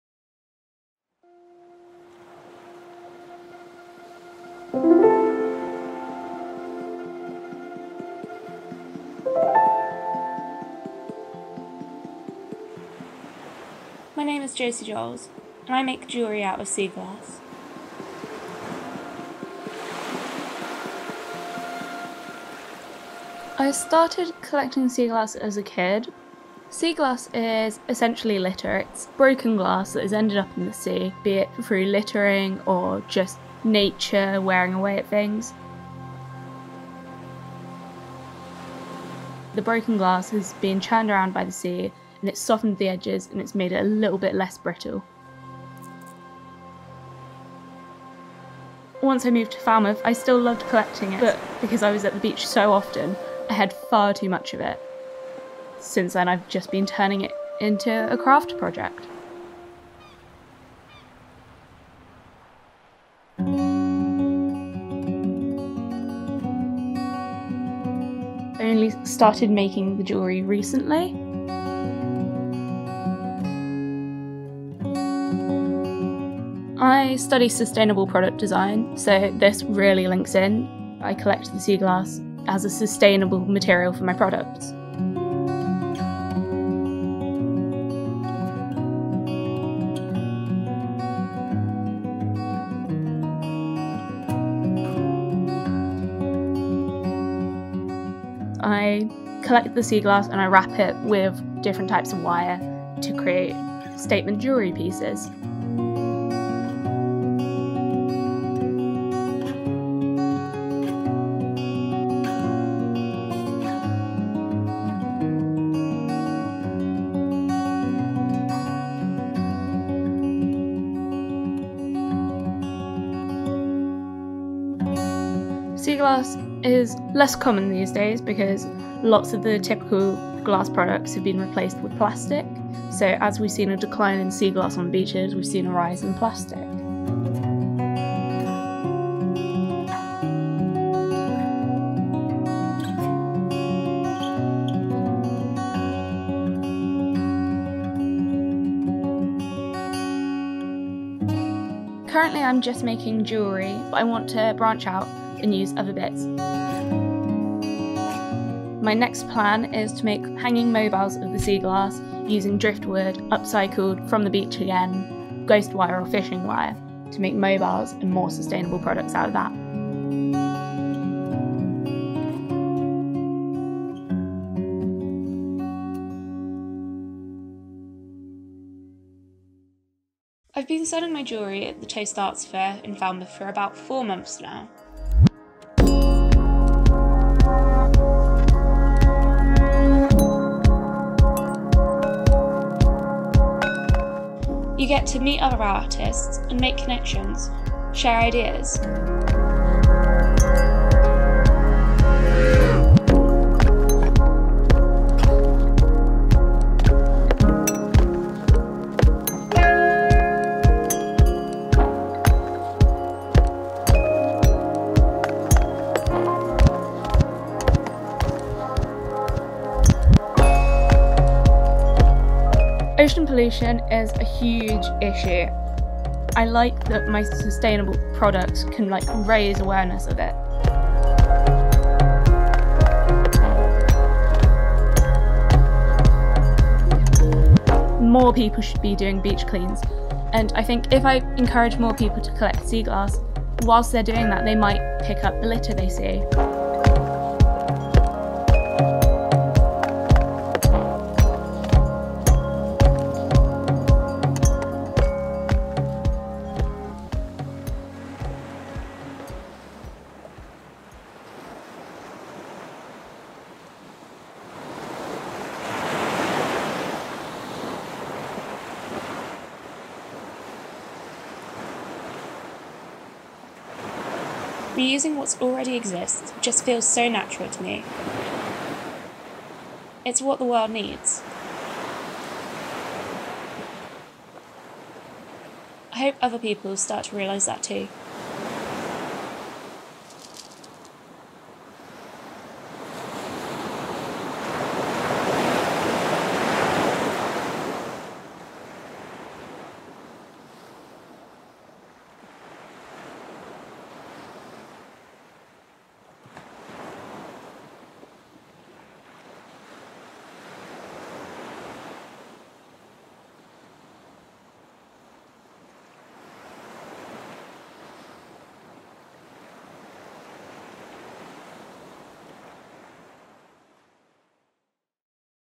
Seacraft
Photographer and environmental activist, Natalie Durrant, takes a dive into film making. She follows a story of regenerative art creating unusual jewellery from the sea. The film follows the narrative of one woman and her creative process in the collection and transformation of her medium.

By Natalie Durrant